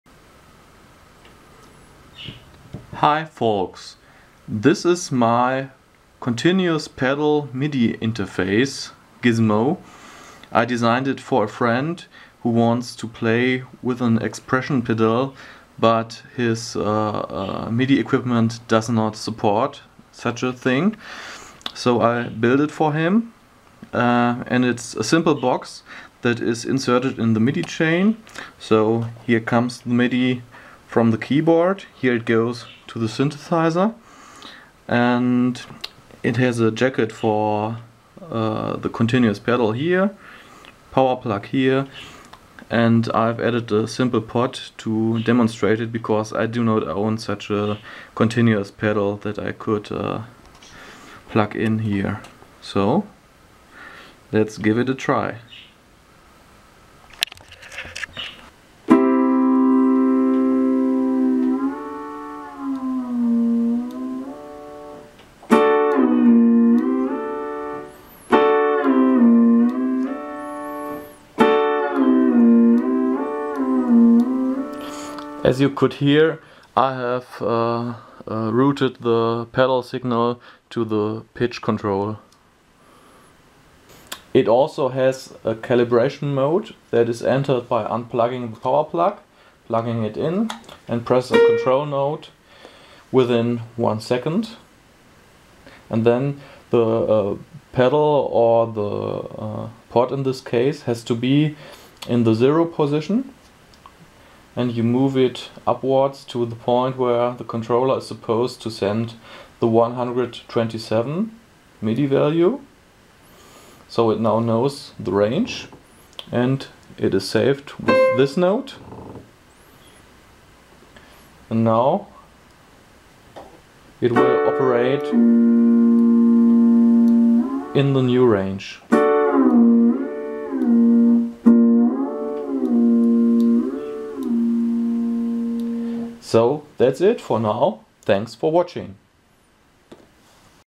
Progressive Pedal Interface Demo
This is a MIDI pedal interface for adding progressive pedal messages to an arbitrary midi stream. It is based on the code for EPiCK.